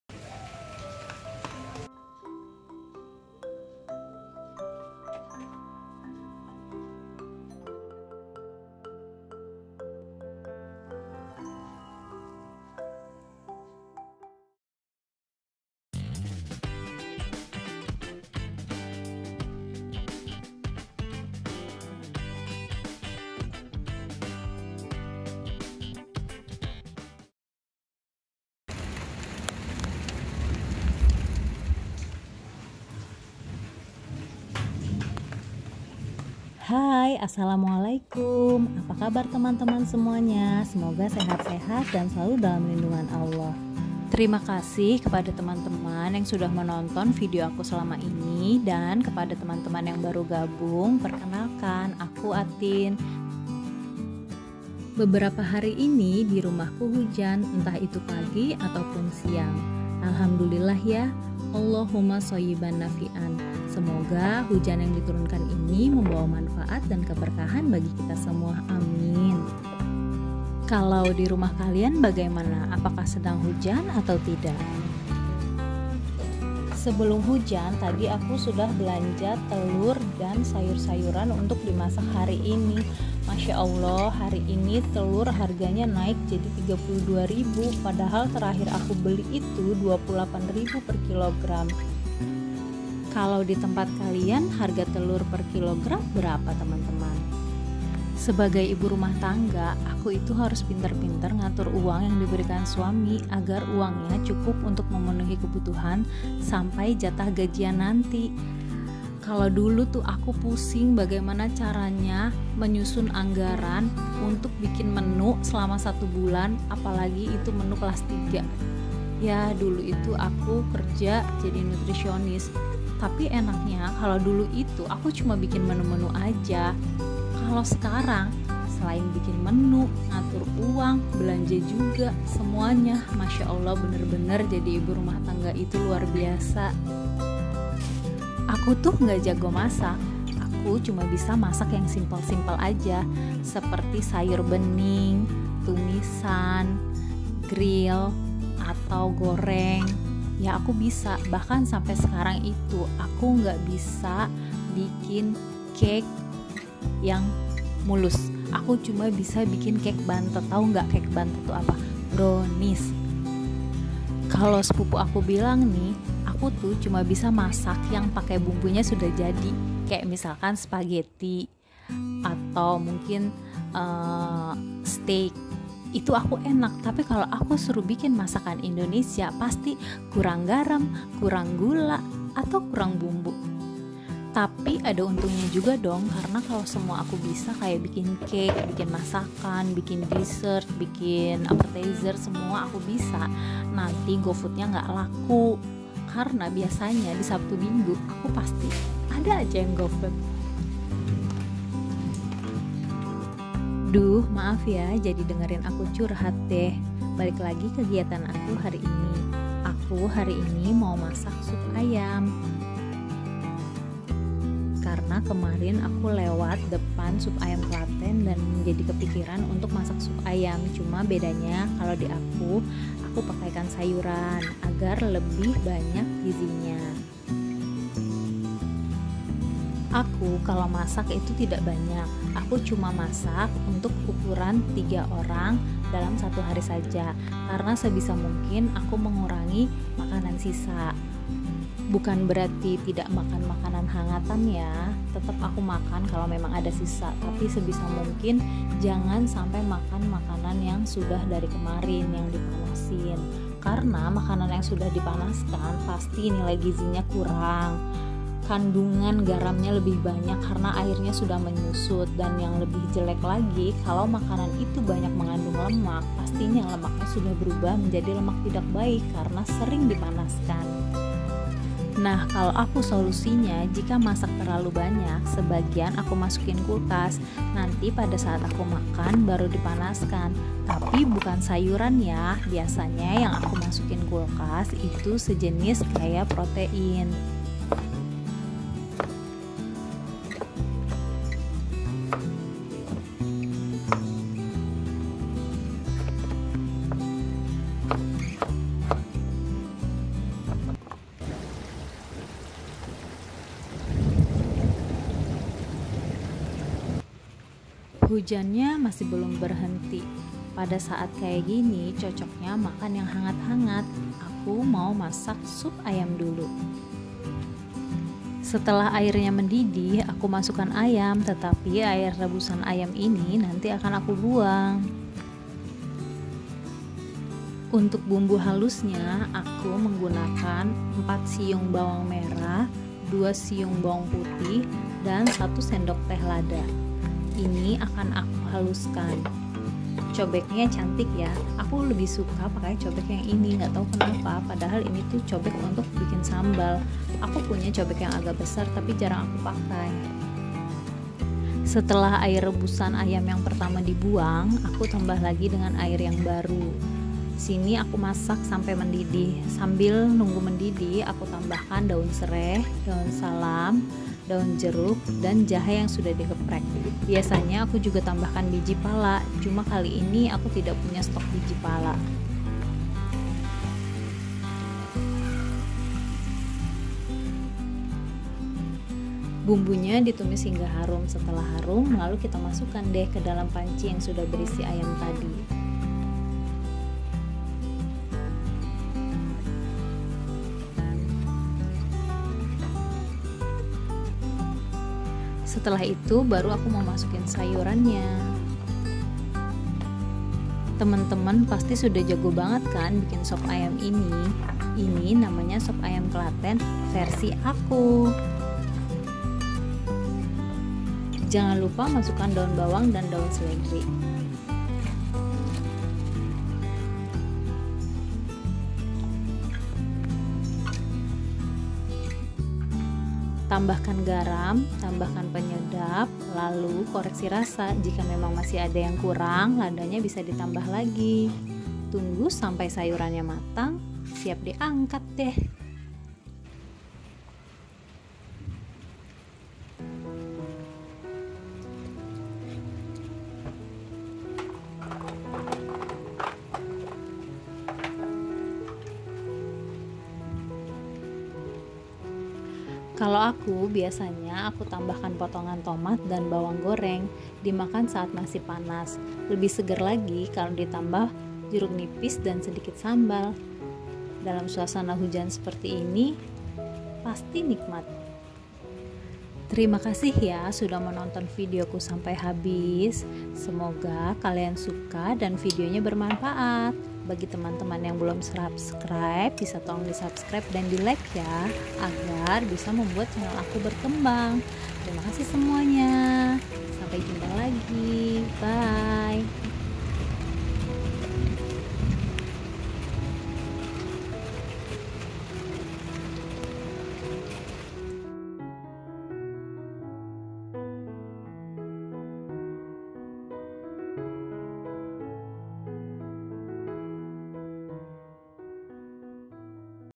dailyvlog Memasak Sop Ayam Klaten Sangat cocok di saat musim hujan
Kegiatan saat hujan memasak sop ayam klaten

Resep
Bahan
Ayam negri/kampung
Sayuran

Bahan halus
Bawang merah
Bawang putih
Lada
Pala bubuk

Bumbu pelengkap
Jahe
Sereh
Daus salam
Daun jeruk

Camera :M62
Edit : Inshoot, Canva.